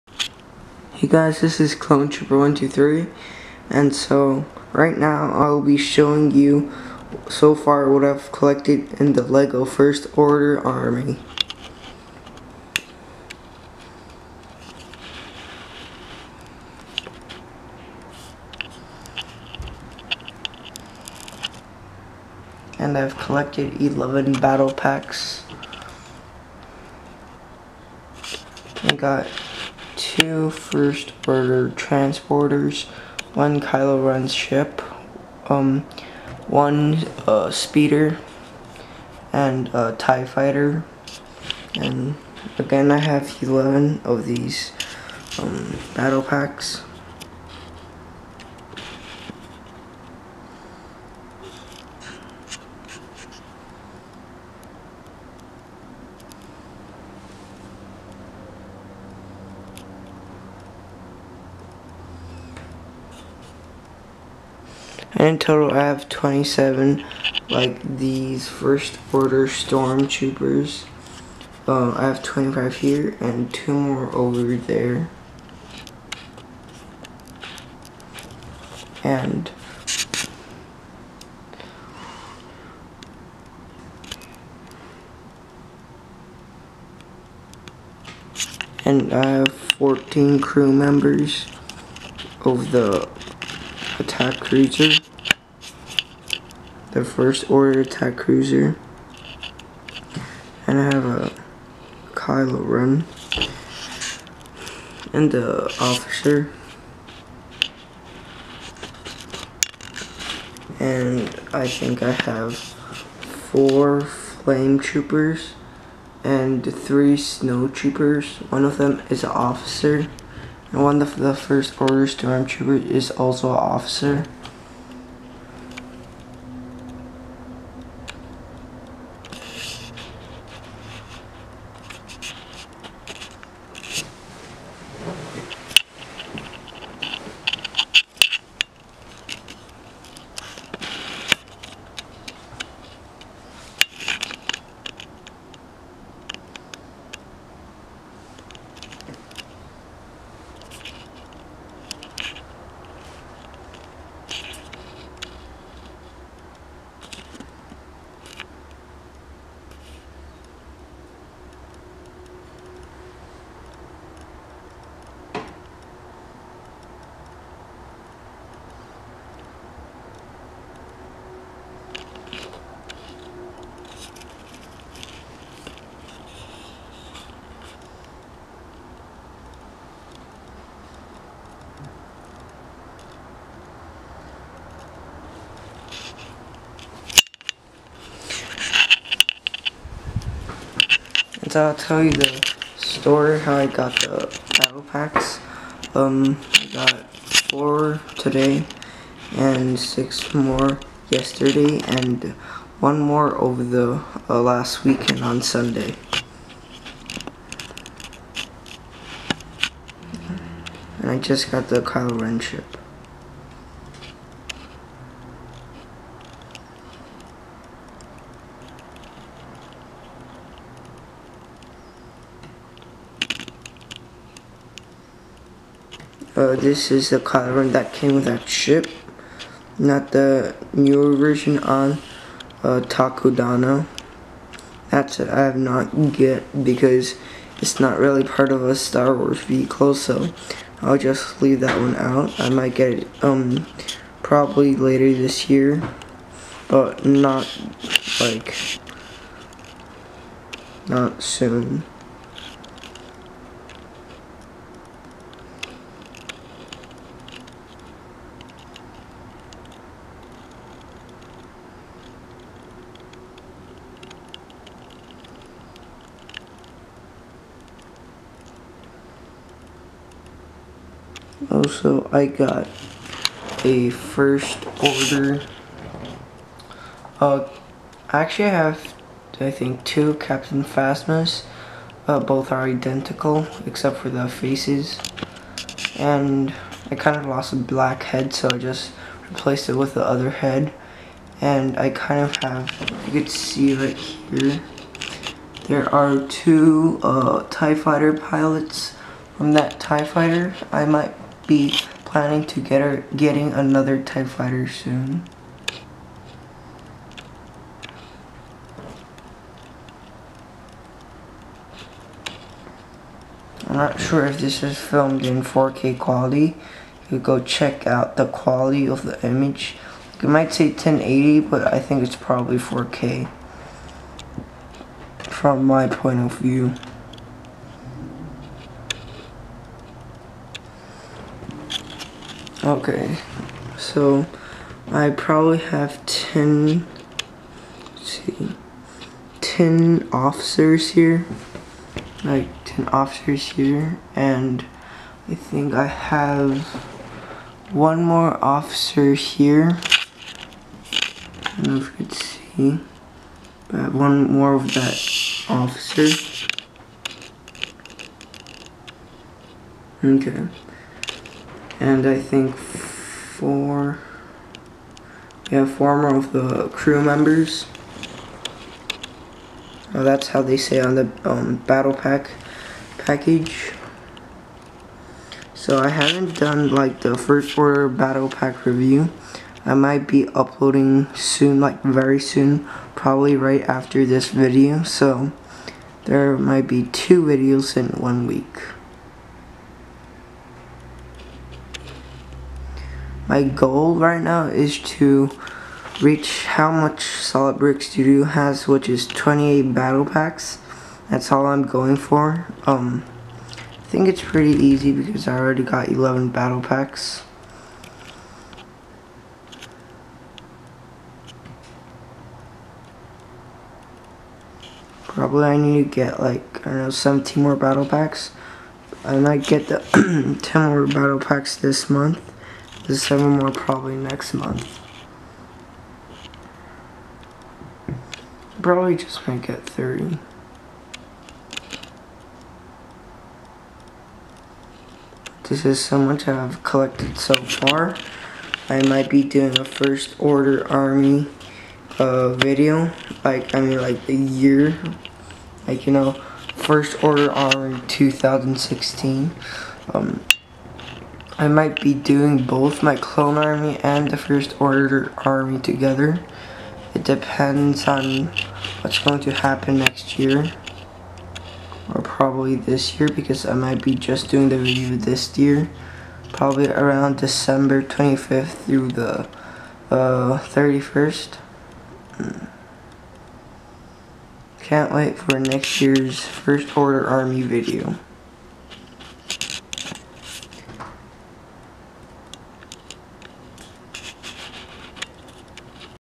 Lego First order army update #1
hey guys so i just bought 11 new batttle packs and built it and make you guys look at the result of the collection i have so far for my first order army.